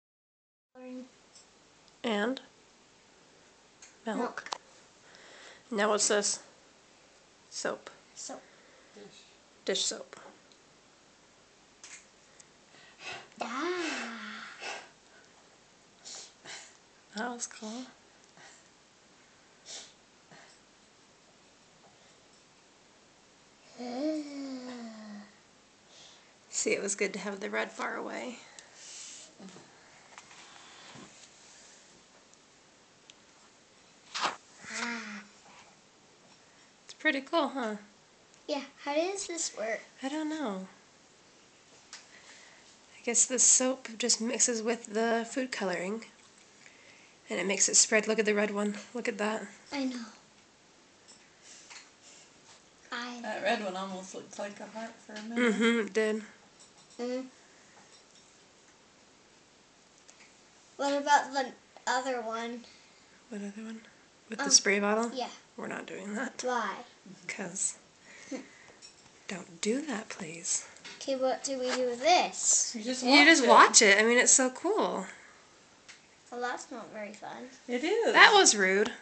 Science Experiment Milk & Food Coloring
What you need: Pie Pan, 1/8 cup milk, 4 drops of food coloring, 1 drop of dish soap (dawn)